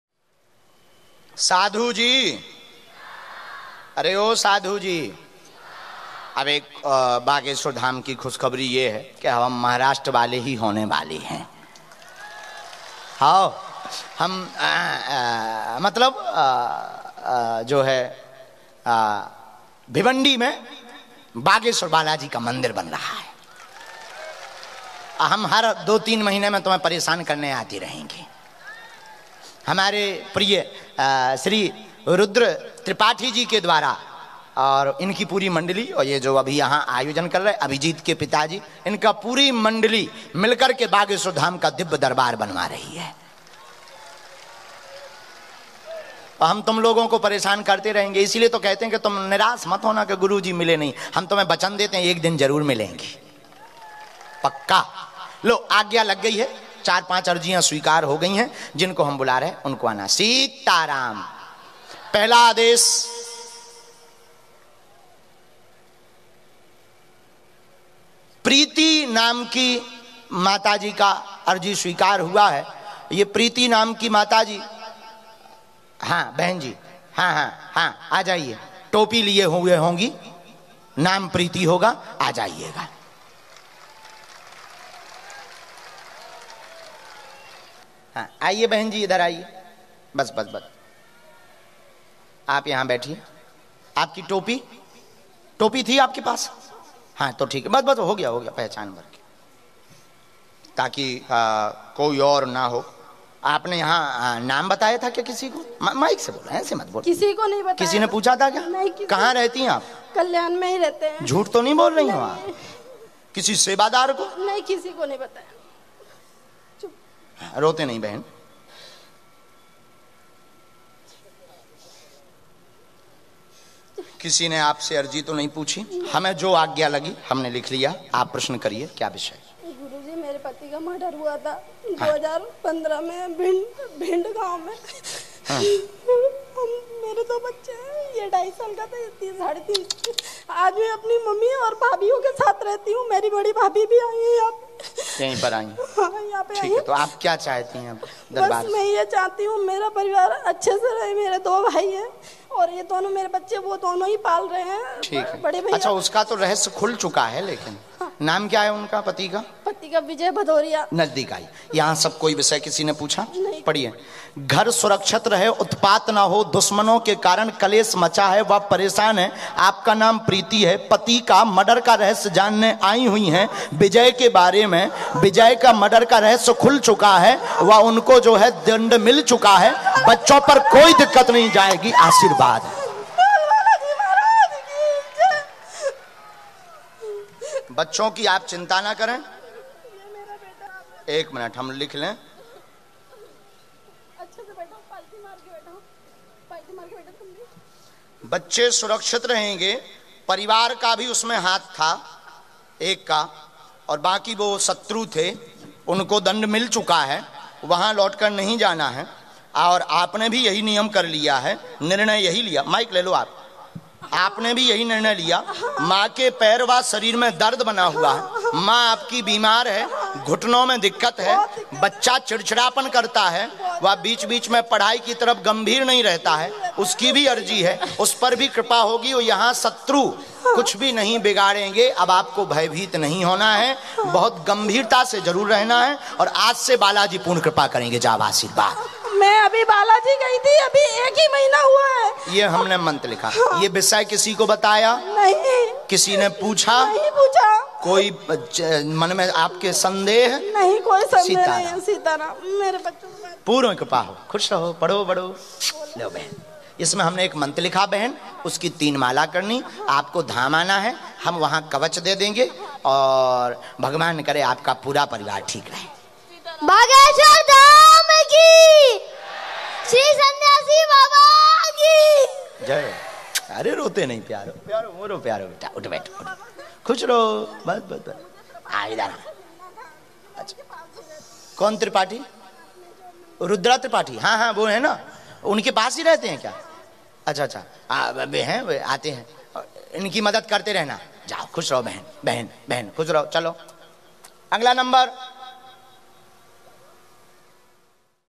घर सुरक्षित रहे उत्पात ना हो ऐसी अर्जी लेकर आई युवती | दिव्य दरबार
Official Detail of Bageshwar Dham Sarkar